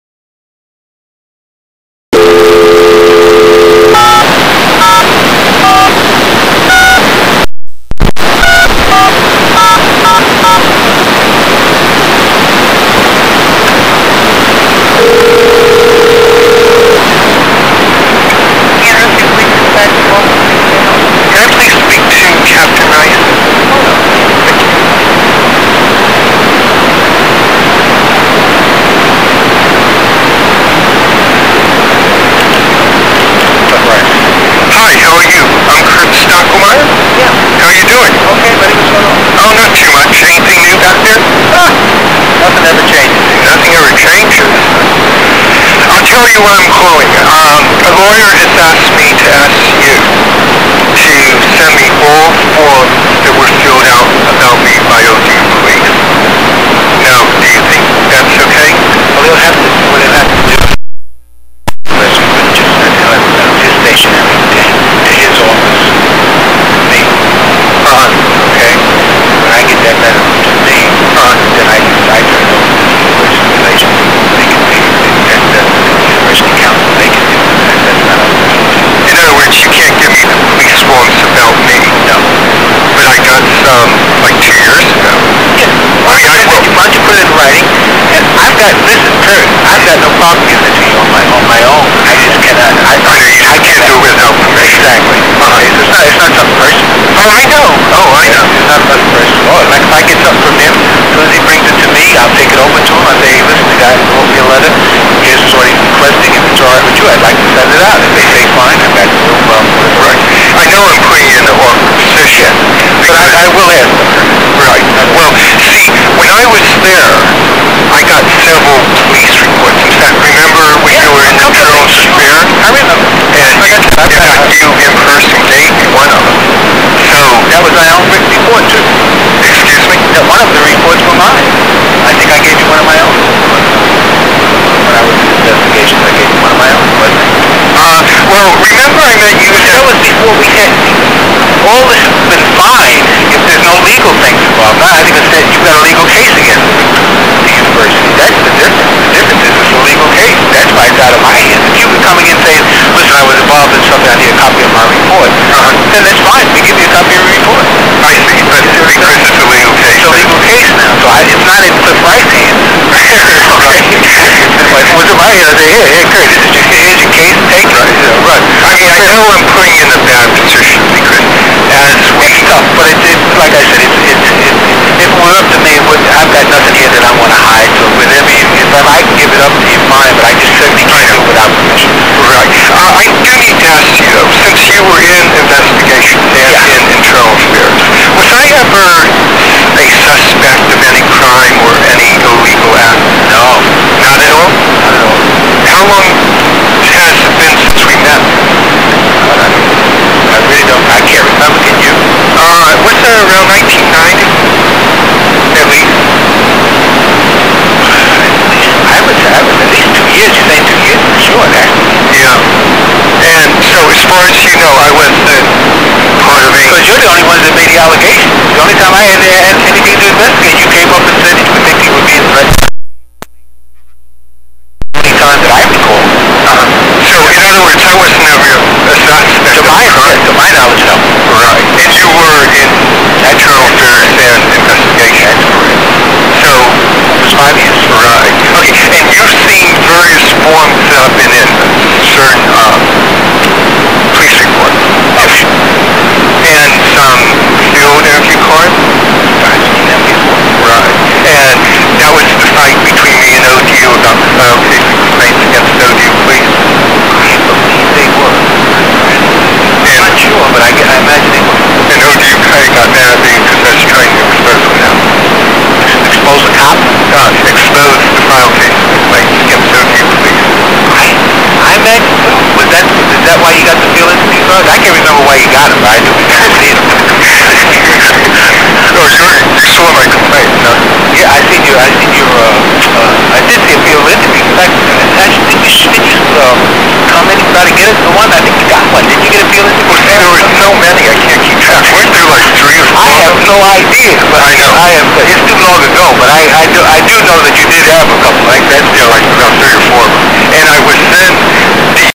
dishonest ODU police
For hours police were taped using a tape recorder.  People who have run Old Dominion University and Old Dominion University police have done thousands of crimes.  I would like good people to look at my video many times to increase number.  It is probably true replay does not increase number.  Good people will not use replay.  For information about me people can look at my page using my link.